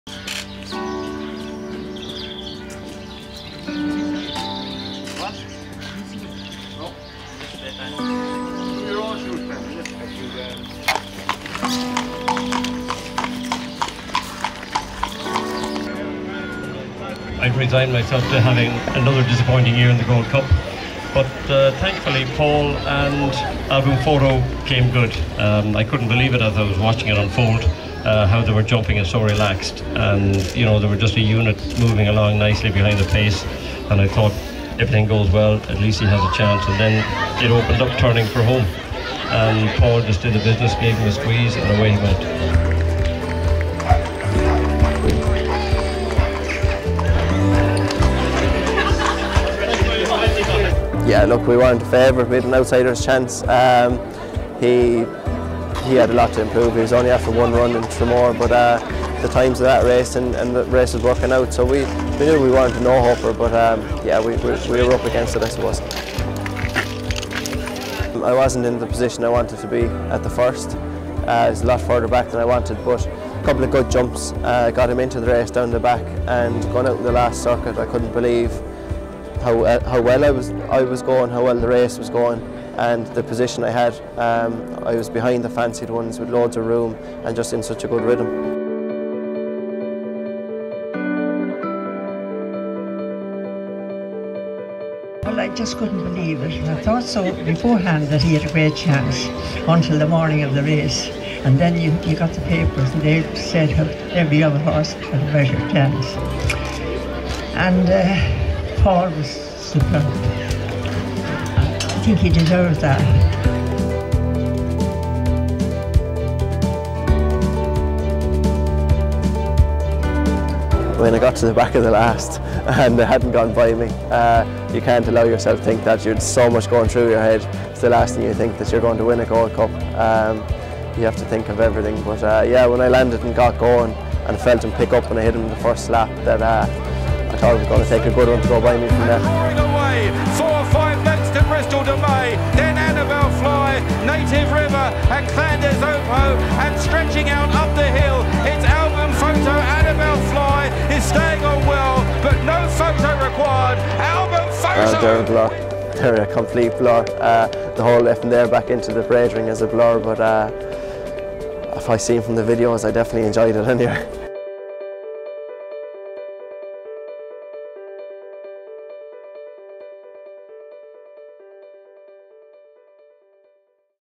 Parade for Gold Cup hero Al Boum Photo in Leighlinbridge
We went down to Leighlinbridge, Co.Carlow where the locals came out to celebrate Gold Cup hero’s Al Boum Photo, Paul Townend and Willie Mullins as they paraded up the main street of the town.

We also caught up with Paul to get this thoughts on what has been a hectic few weeks for the Gold Cup winning jockey.